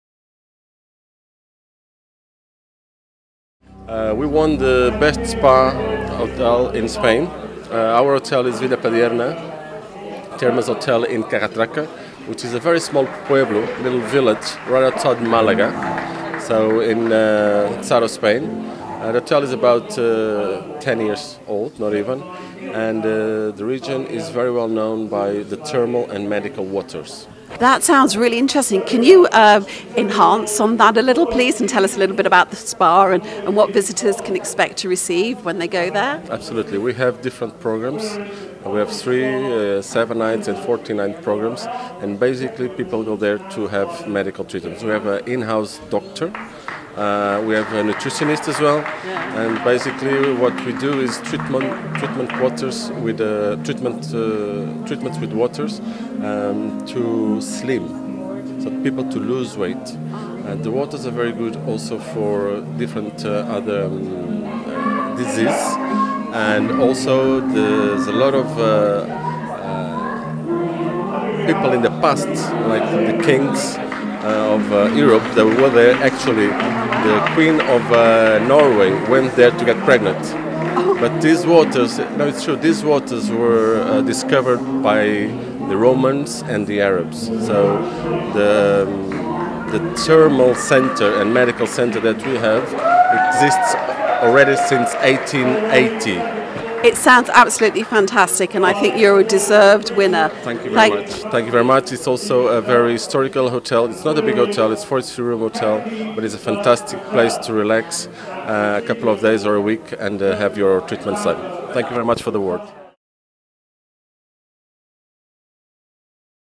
Jorge de Almeida, General Manager, Hotel Villa Padierna Thermas Carratraca
Jorge de Almeida talks to Breaking Travel News at the World Travel Awards Europe Gala Ceremony 2015